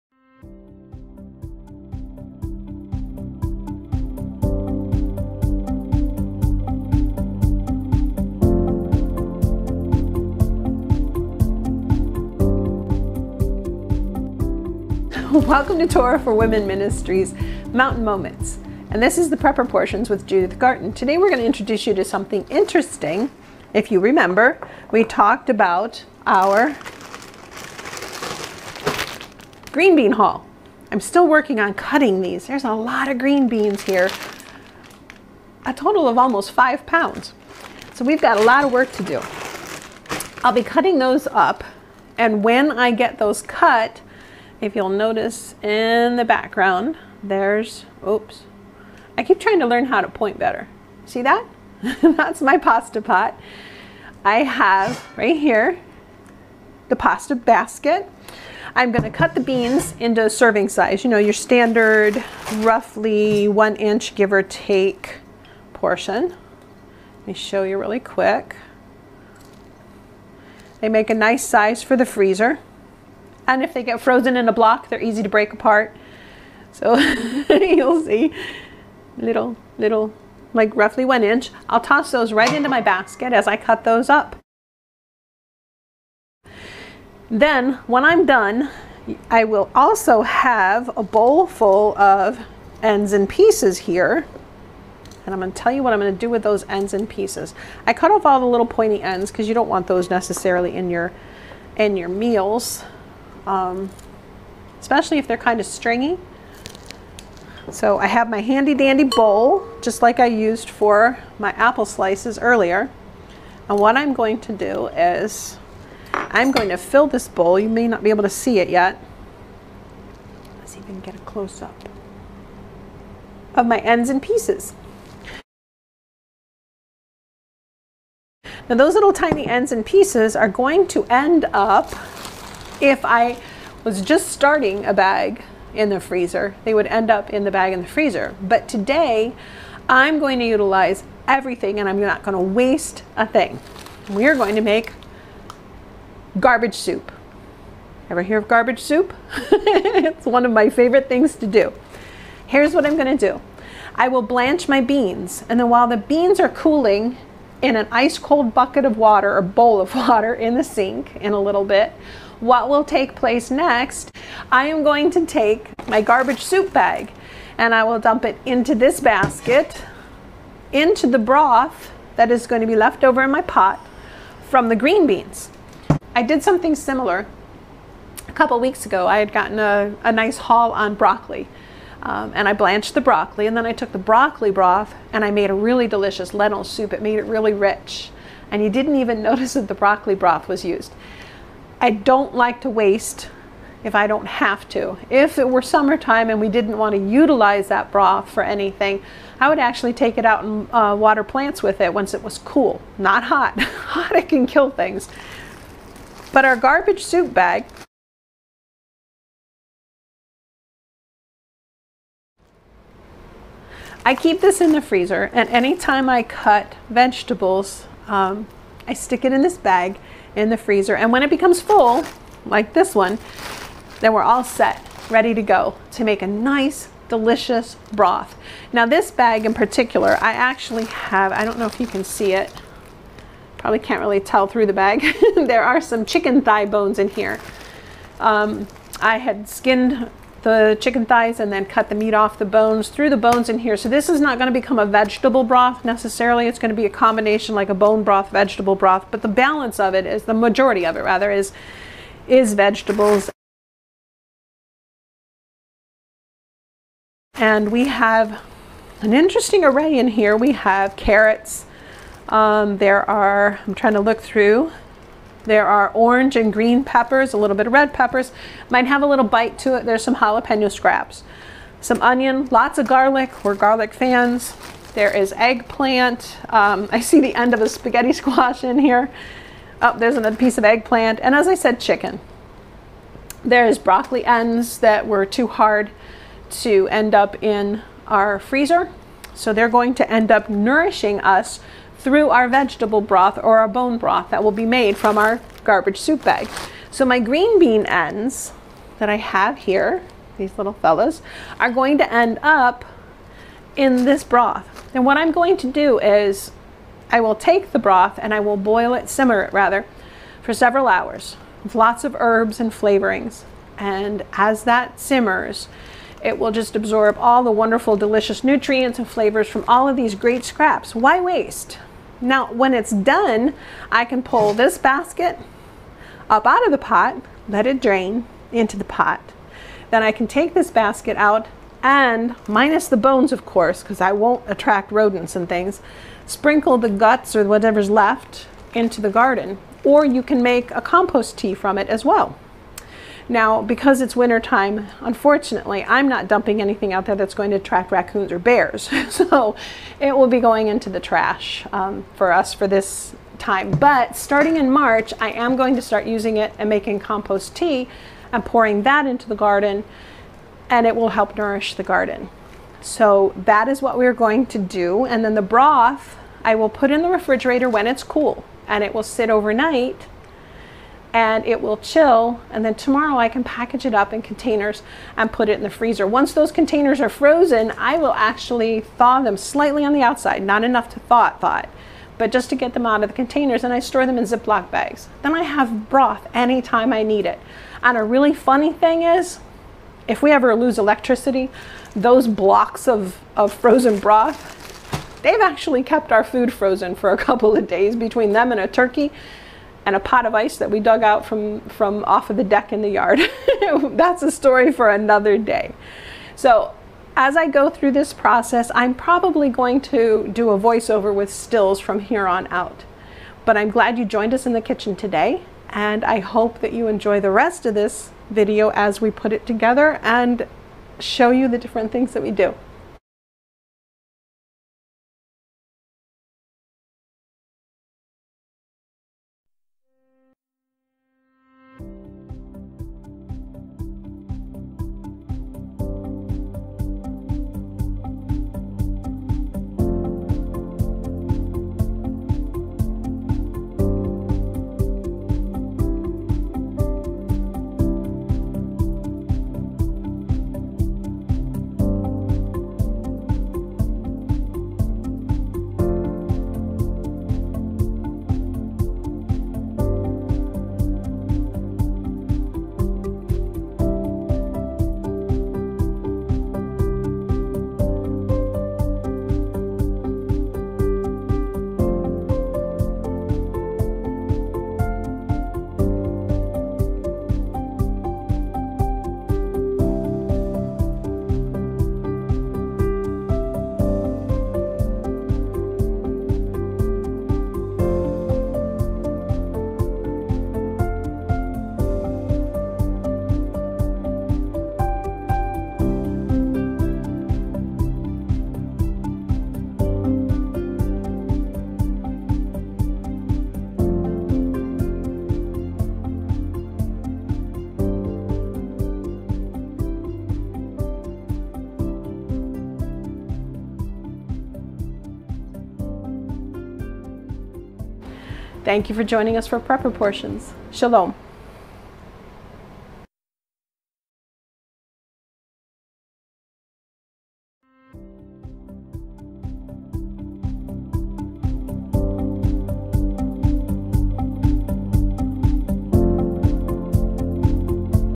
Garbage Soup
I have seen a LOT of recipes for "Garbage Soup" or "Garbage Stew" - and they look yummy. However, MY recipe is not quite as edible and pretty as those recipes are. Want to learn why? Check it out - and you may never have to buy a common pantry ingredient ever again.

Keep your eyes out for our new and improved book coming soon!